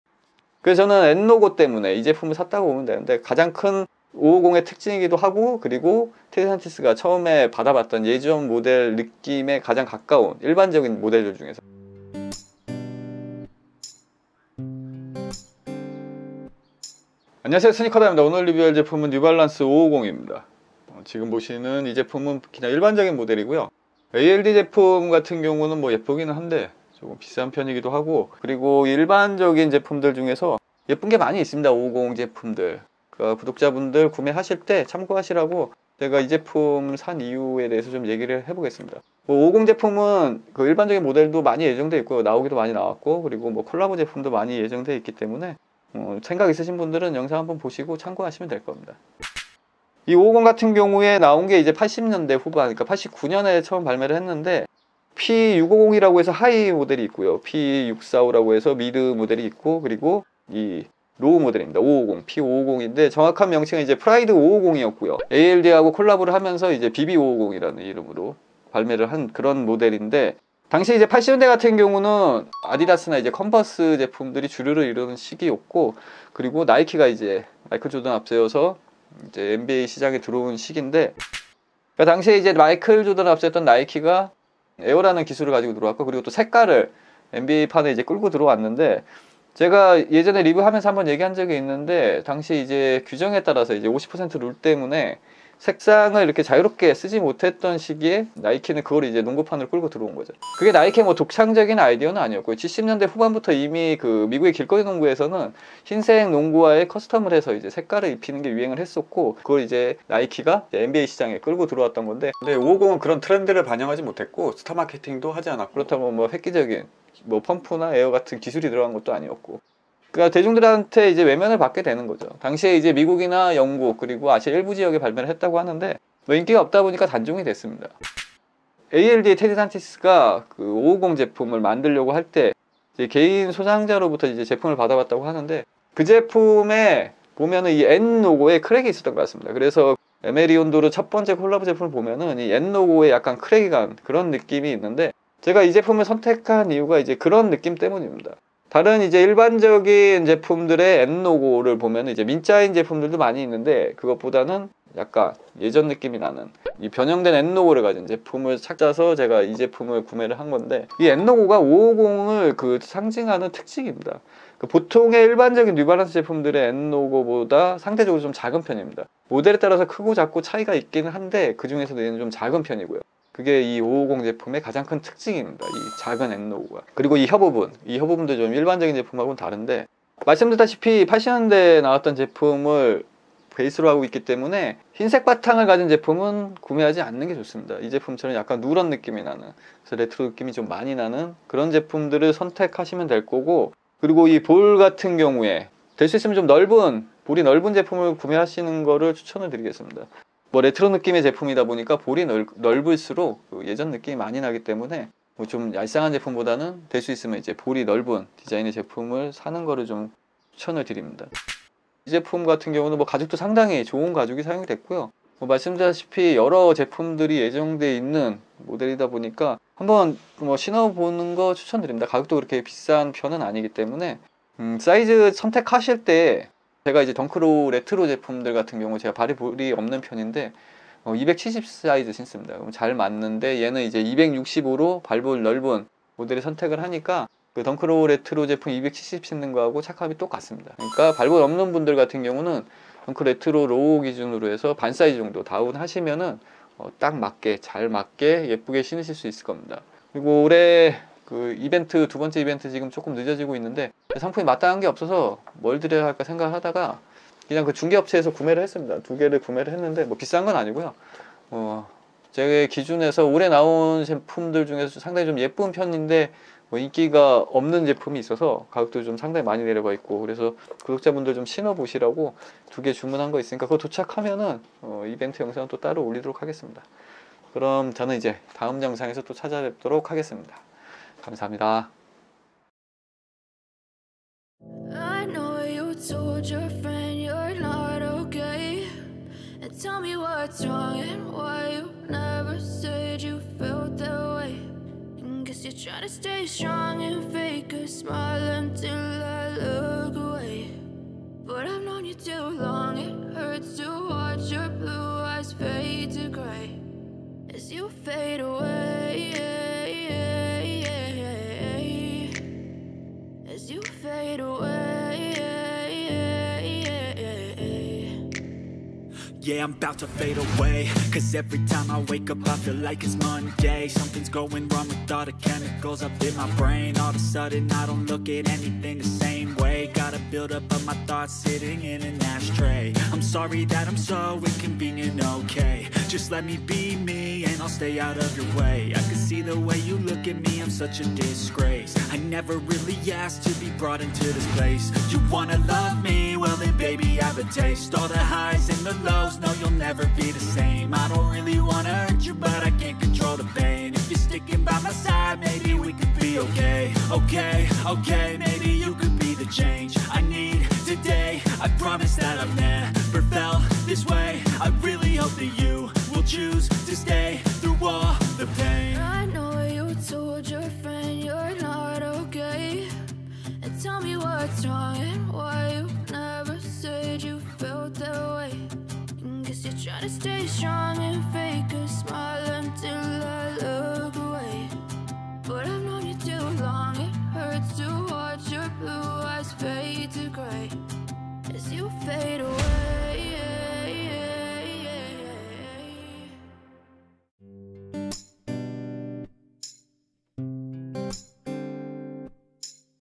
수많은 550 중 어떤 걸 사야 할까? (요즘 신발 뭐삼? #50)
요즘 신발 뭐삼?
New Balance 550 White Grey - 2E Wide

뉴발란스 550 화이트 그레이 - 2E 와이드

BB550LWT

it's AUTHENTIC !